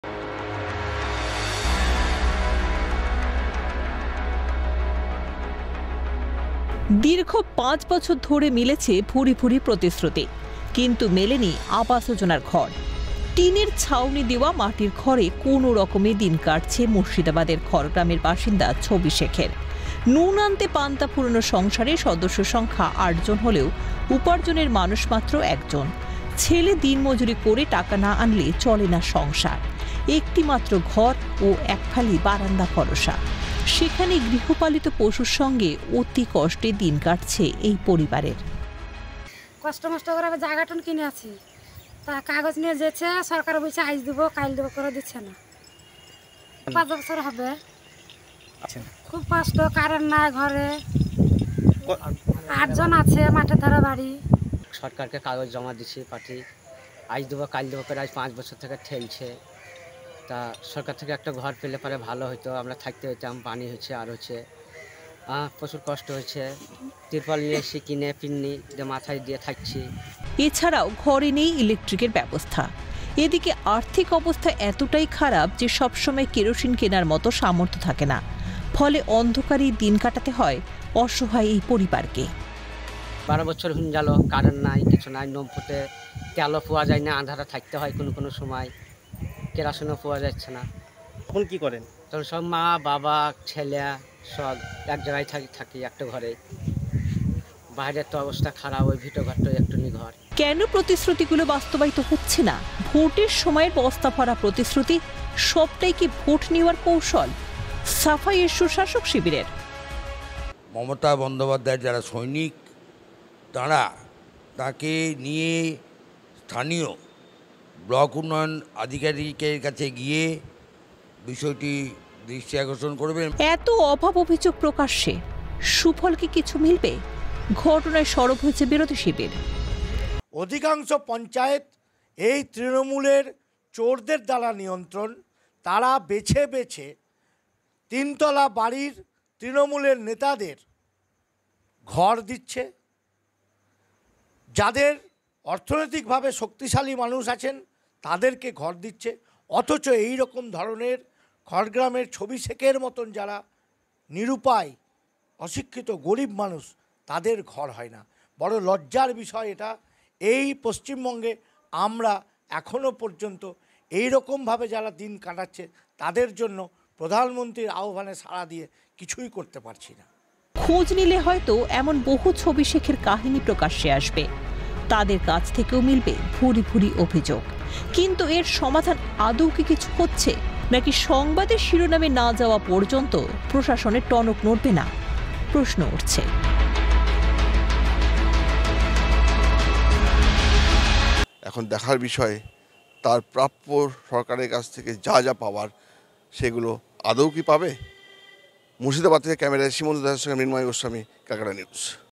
অধরা আবাস যোজনার স্বপ্ন, ৫ বছরের 'প্রতিশ্রুতি' মিথ্যা? | ক্যালকাটা নিউজ
অধরা আবাস যোজনার স্বপ্ন, ৫ বছরের 'প্রতিশ্রুতি' মিথ্যা?

মুখেই প্রতিশ্রুতির বন্যা। বাস্তবে সবটাই ফাঁকা। আবাস যোজনার ঘর না পেয়ে একরাশ ক্ষোভ মুর্শিদাবাদের খড়গ্রামের একটি পরিবারের। আদৌ কি মিলবে ঘর? আশায় রয়েছে অসহায় এই পরিবার।